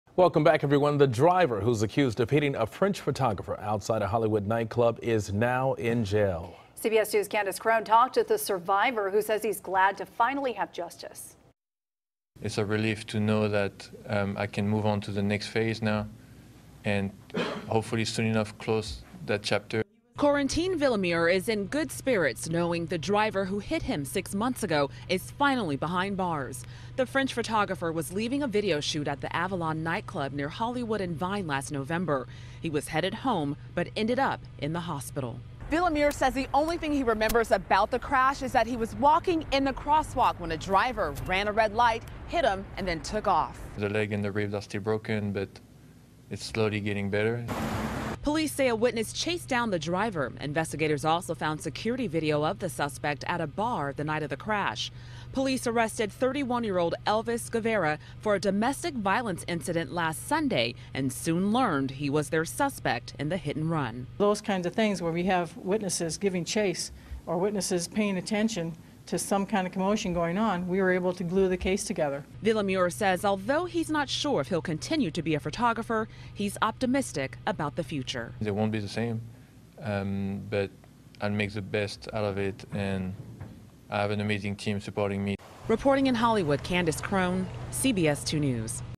Arrest Made In Hit-And-Run Crash That Injured French Photographer
Photographer Corentin Villemeur was leaving a video shoot in Hollywood last November when he was struck by a car. Kandiss Crone reports.